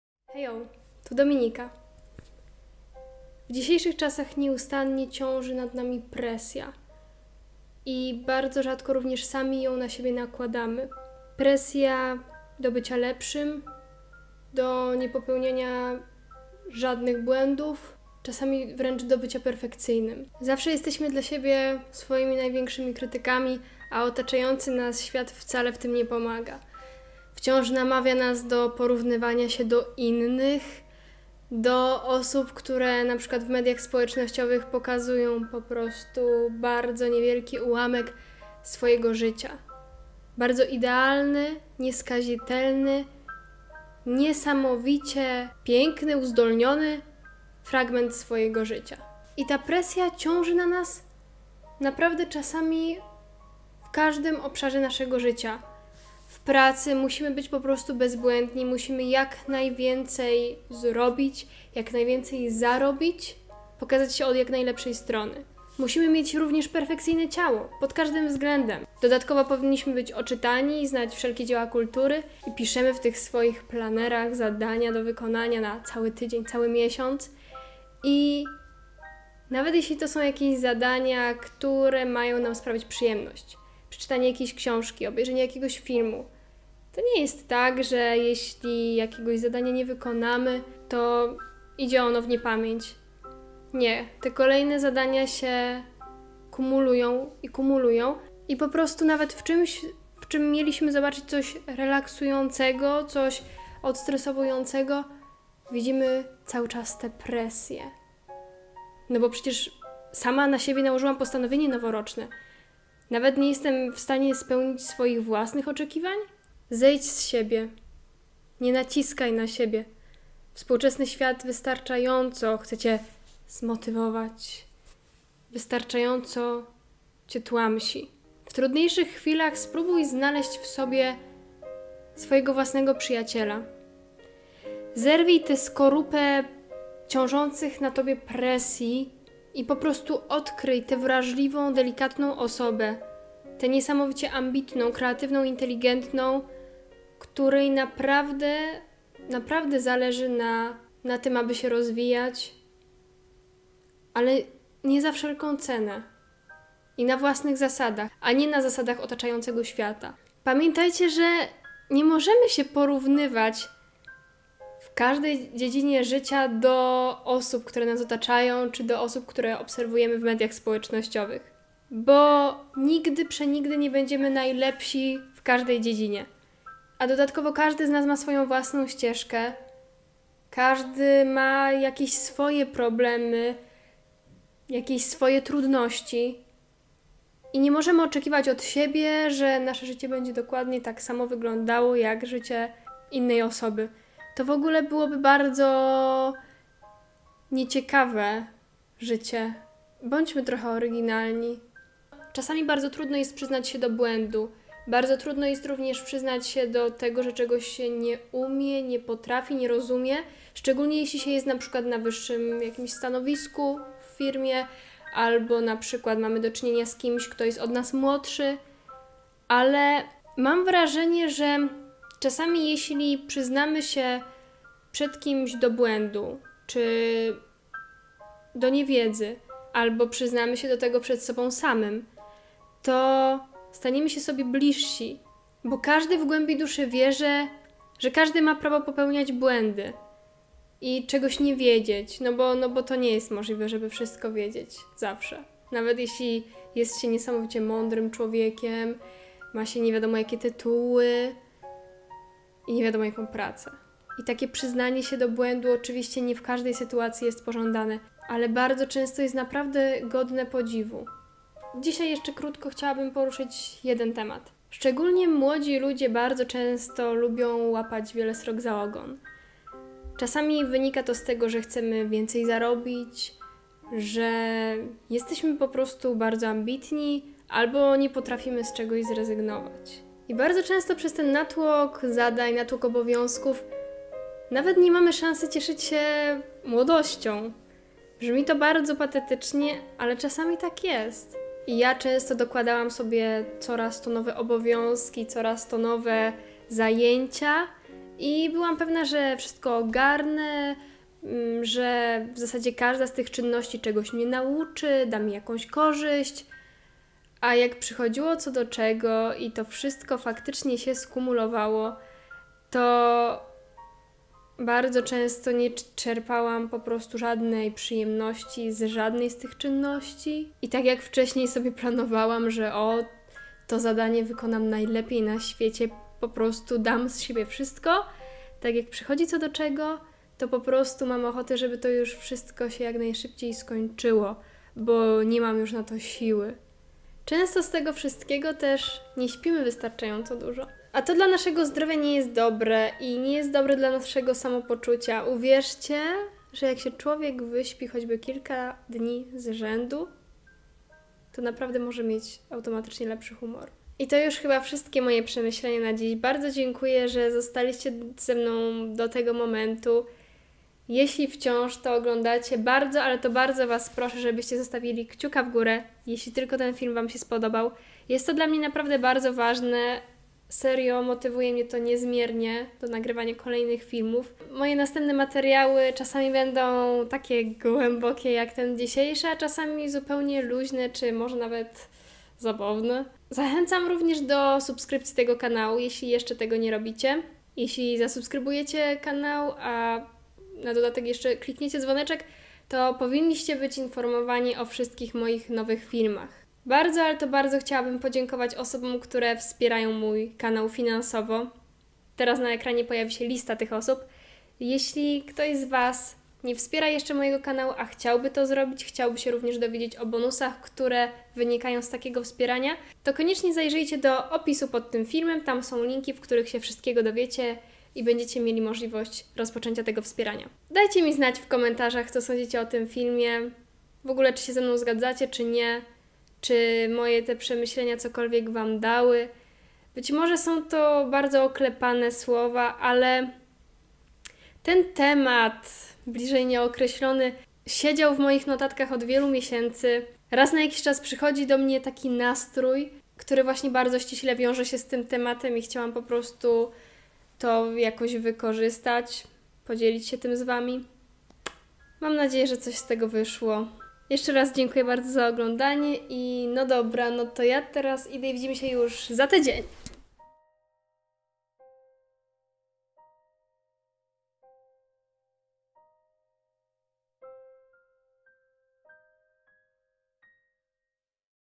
Nie potrafię...
W tym filmie dzielę się z Wami moimi ostatnimi przemyśleniami na temat presji społecznej, nie wiporównywania się do innych, przyznawania się do niewiedzy i nie tylko. Zapraszam do oglądania 🙂